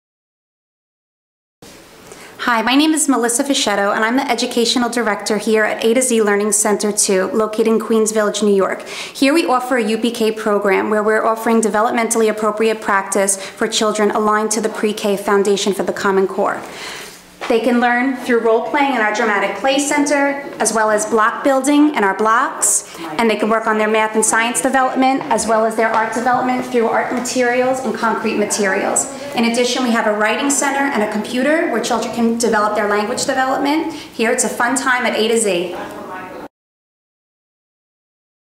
A To Z Pre Kindergarten Program
tour of the A  to Z Pre k school in Middle Village Queens.